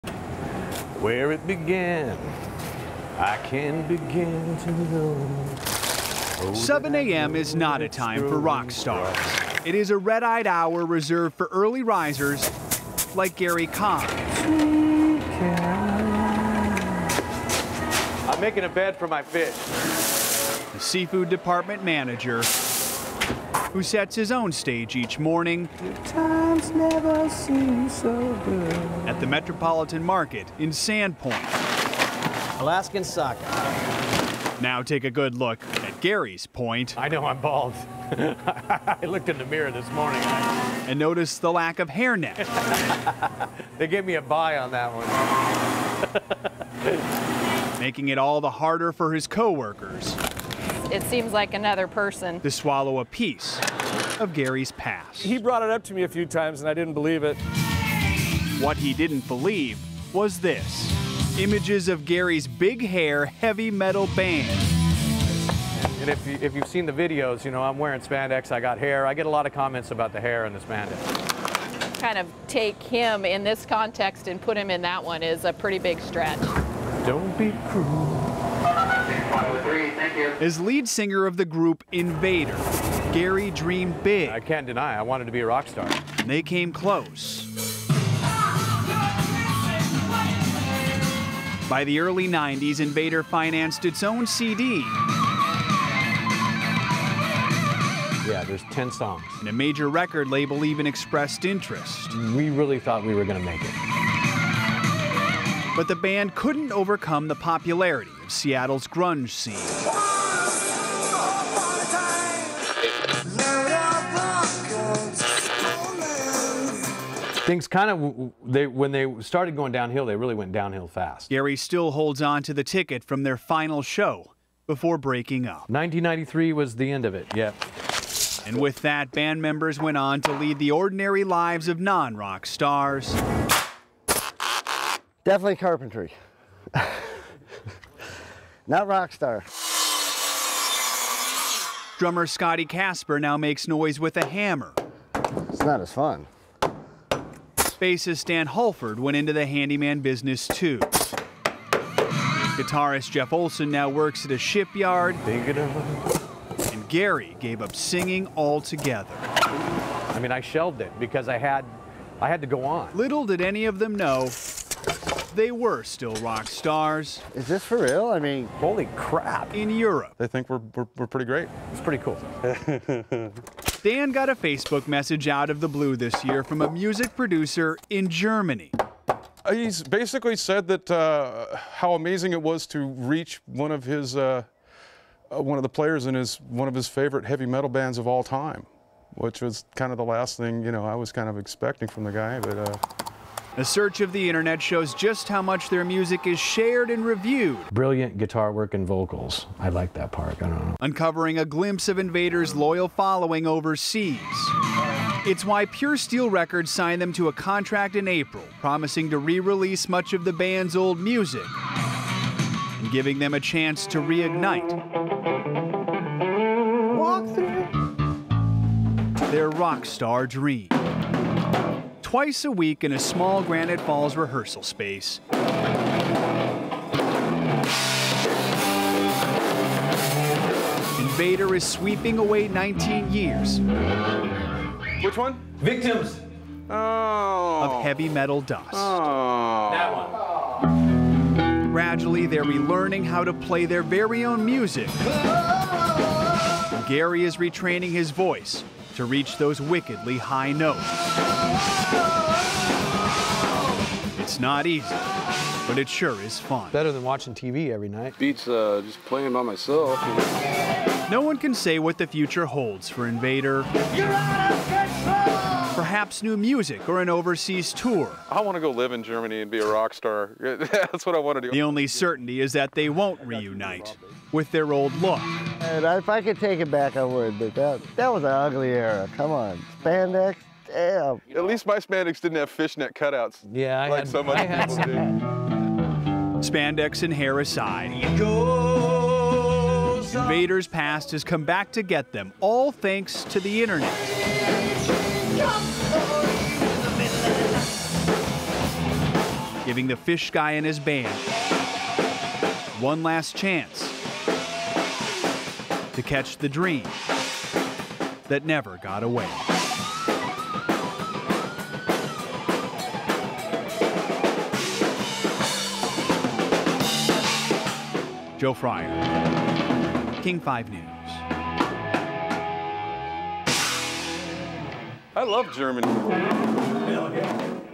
Heavy Metal Dust (10 p.m. version)
In-depth feature story on Invader, a Seattle heavy metal band that broke up 20 years ago, only to receive a surprise this year.